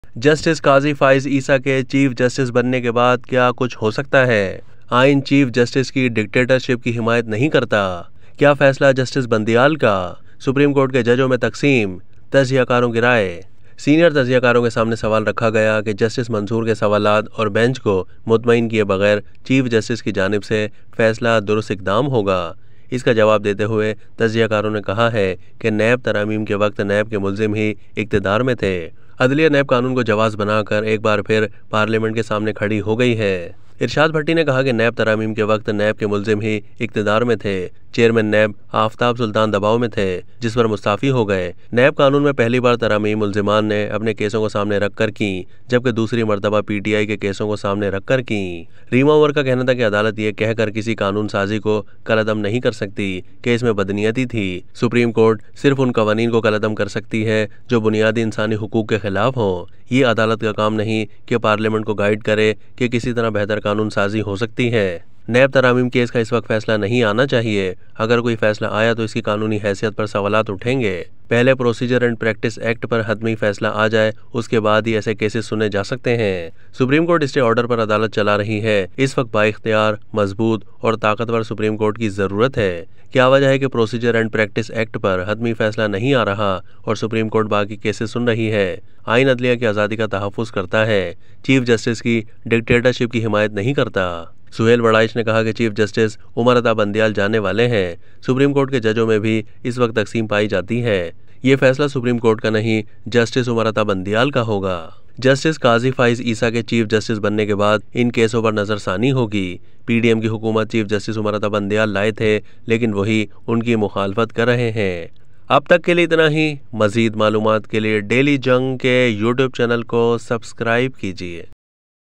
What Could Happen After Qazi Faez Isa Become Chief Justice Of Pakistan?
Suhail Warraich said that Chief Justice Umar Ata Bandial is going to leave, and there is a division among the judges of the Supreme Court.